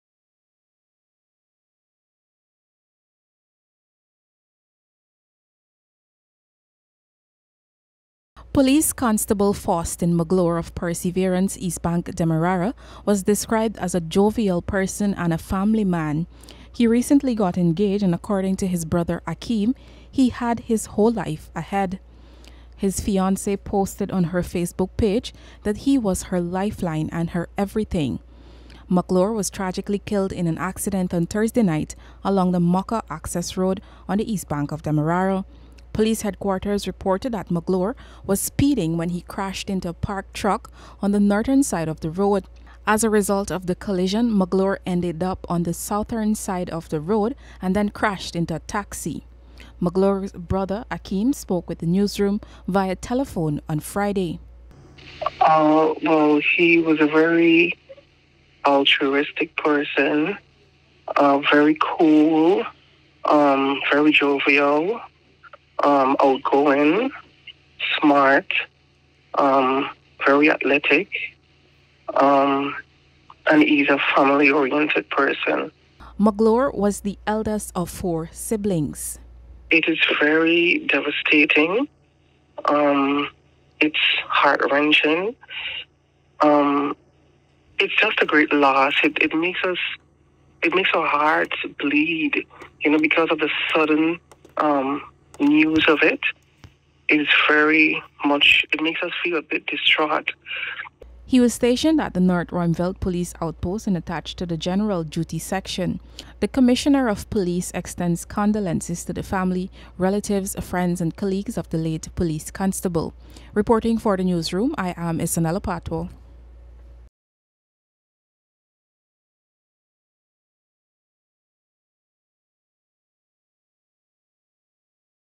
RECENTLY ENGAGED COP KILLED IN EBD ACCIDENT
The Guyana Police Force is mourning the loss of 27-year-old police constable Faustin Magloire who was killed after he collided with a parked truck along the Mocha Access Road, East Bank Demerara (EBD), on Thursday night. Police said Magloire was speeding at the time of the accident.